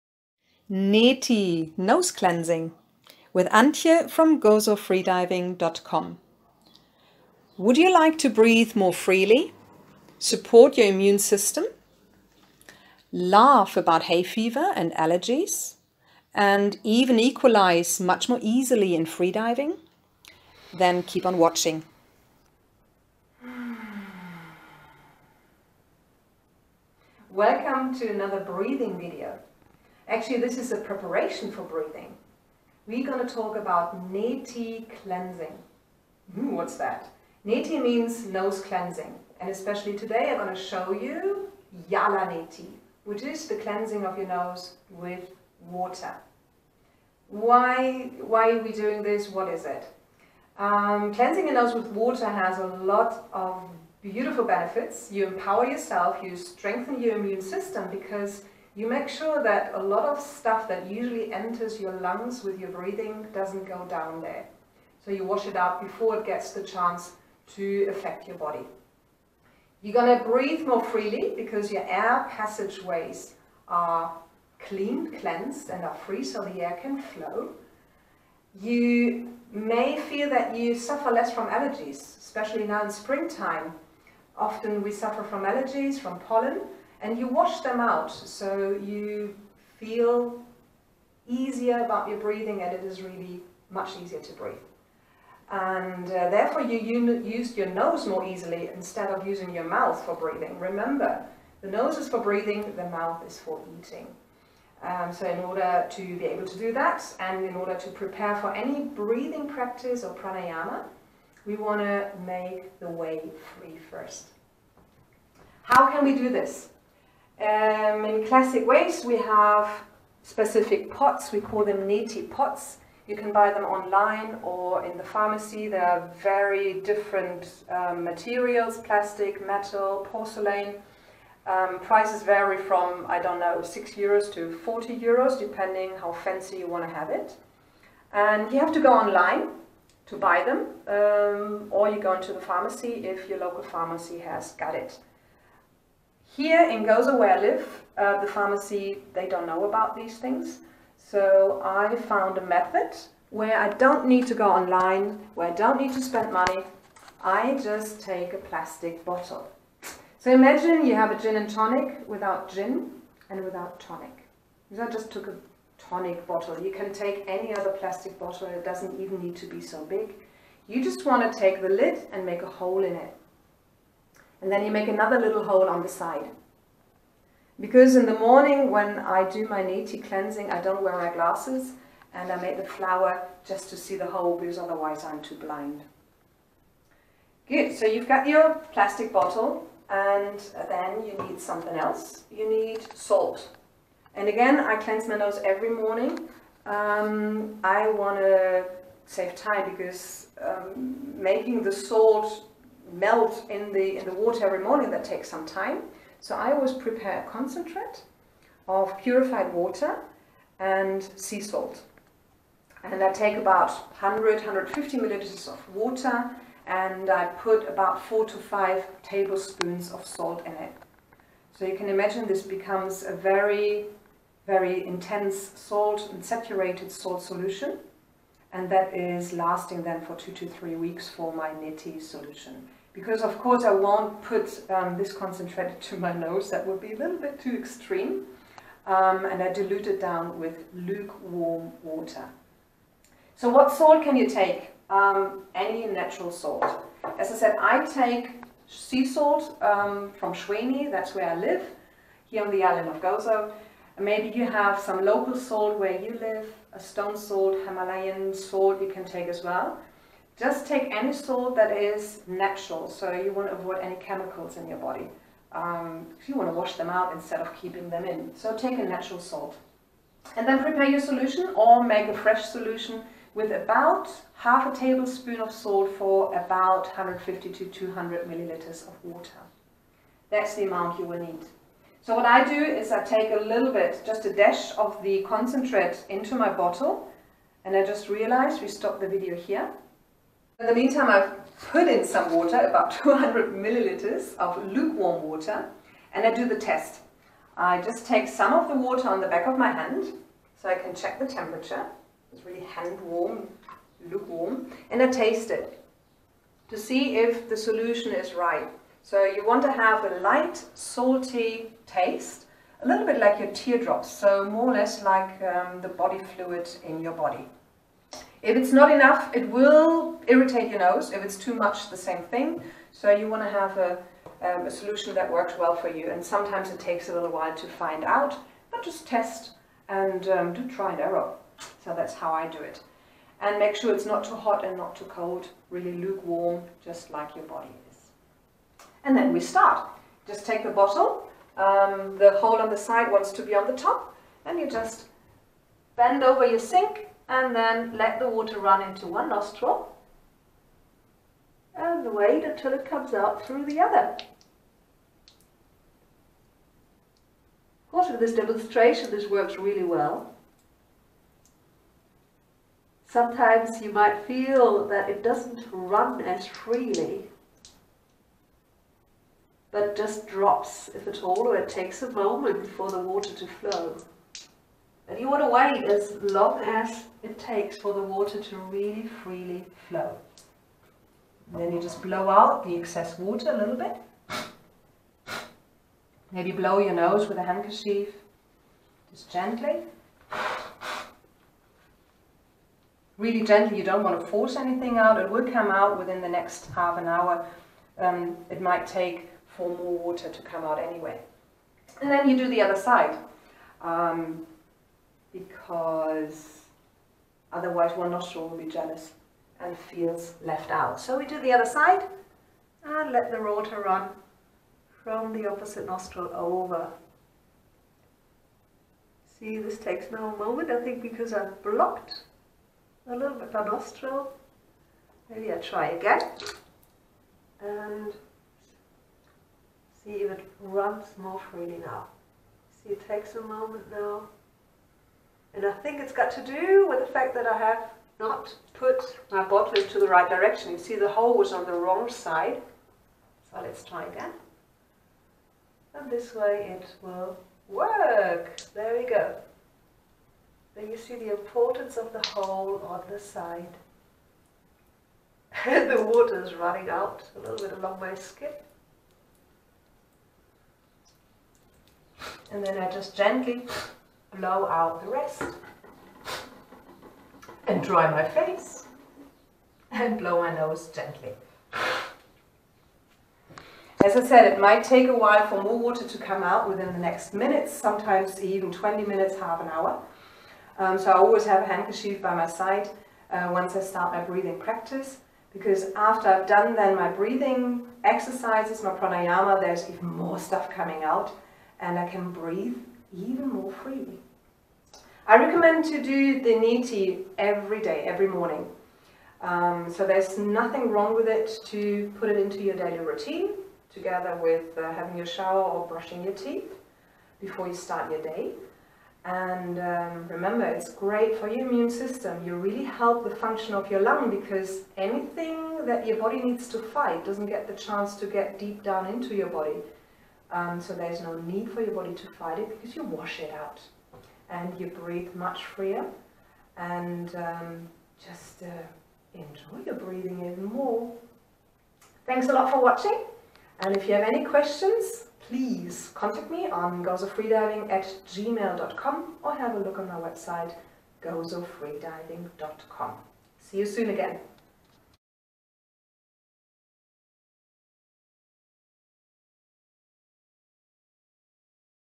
Neti - Nose Cleansing
Help your lungs to do a great job in keeping you healthy
Support your immune system
Breathe more easily
Cleanse your sinuses
Laugh about allergies
Equalise more easily in freediving
Neti is much more than just blowing your nose.
And it's easy.
You just need to do it. Daily.